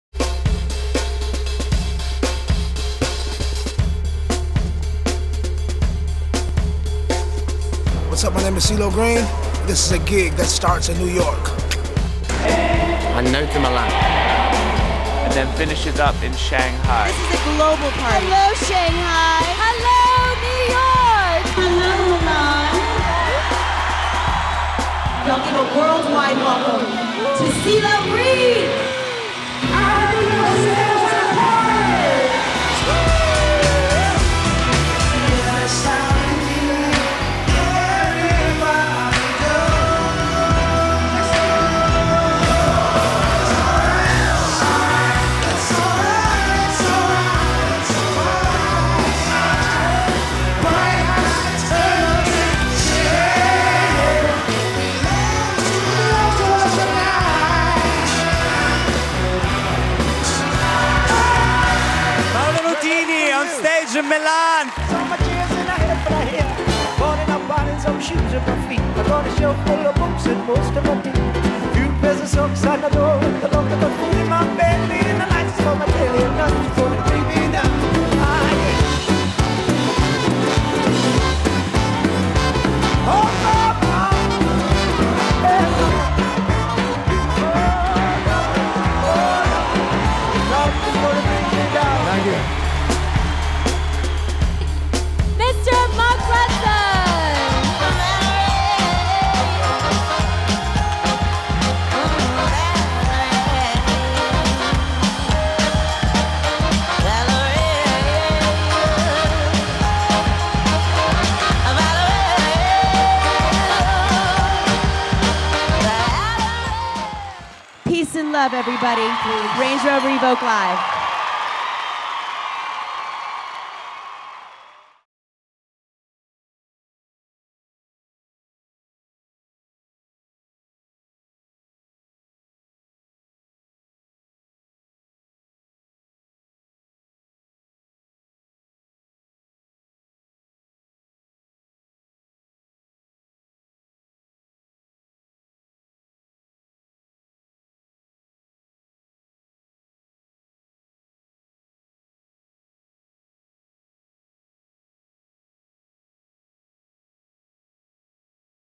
Range Rover Evoque Live Event
The Range Rover live event combines contemporary design with the chance for people to get up close to the Evoque compact SUV; before it goes on sale in late 2011.

Featuring Ceelo Green in New York and then simultaneously, Range Rover Evoque united the world with two additional concerts, featuring performances by Mark Ronson in Shanghai and Paolo Nutini in Milan.